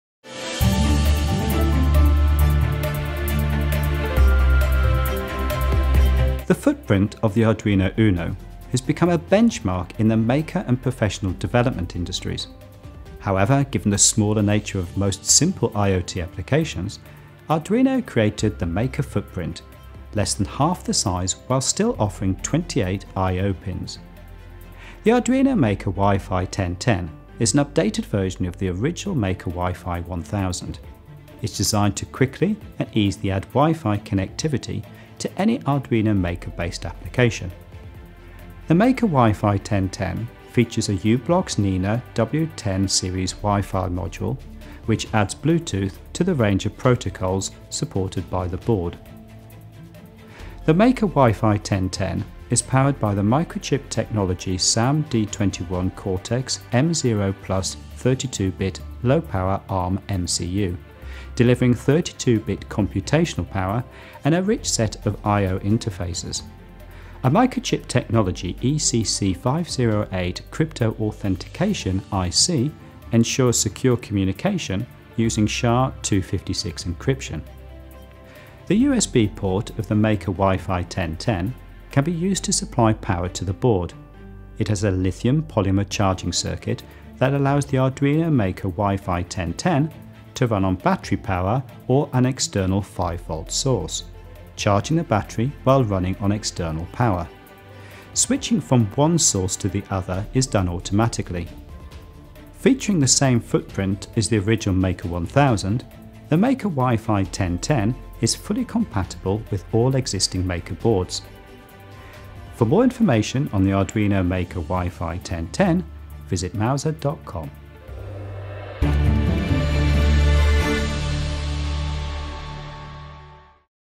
Arduino MKR WiFi 1010 | Featured Product Spotlight
Arduino created the MKR boards for small and simple IoT applications. Its new version, the Arduino MKR WiFi 1010 adds Bluetooth to the supported protocols as well as a range of features.